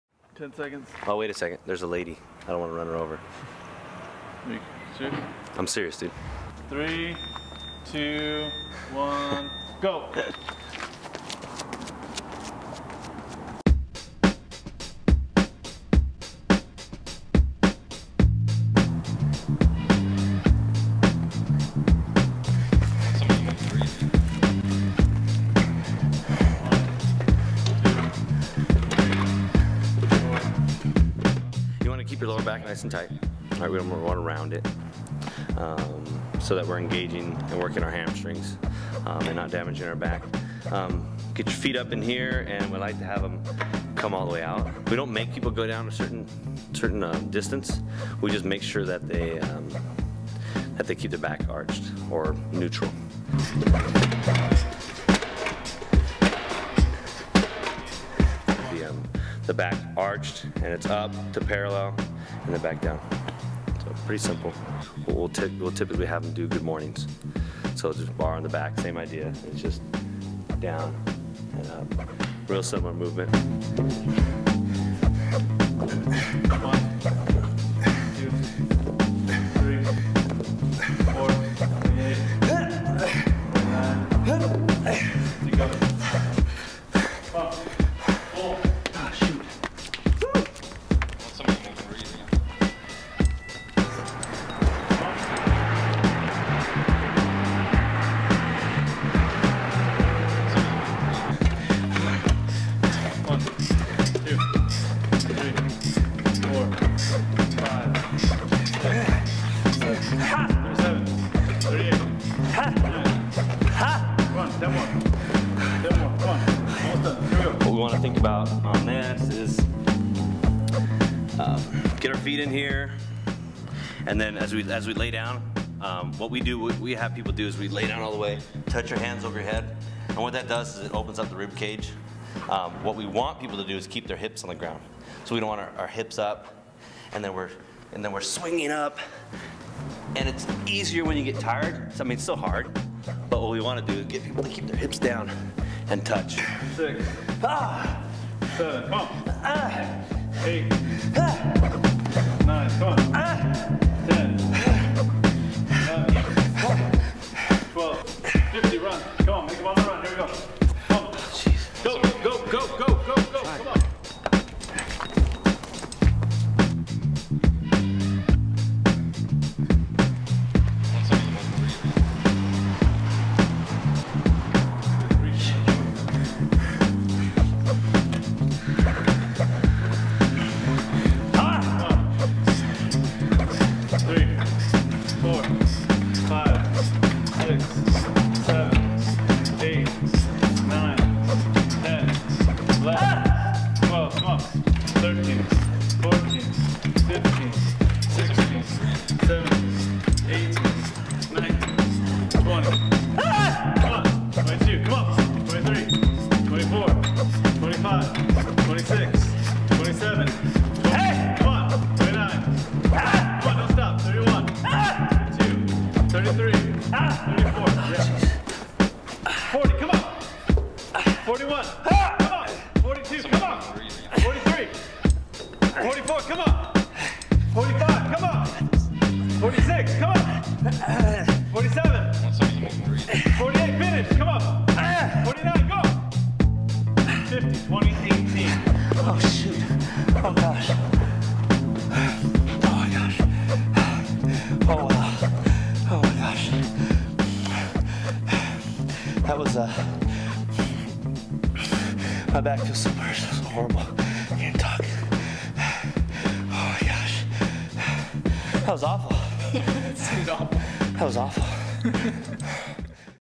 CrossFit - WOD 111119 Demo "Michael" with Jeremy Kinnick
"Michael"

Three rounds for time of:
Run 800 meters
50 Back Extensions
50 Sit-ups

Post time to comments.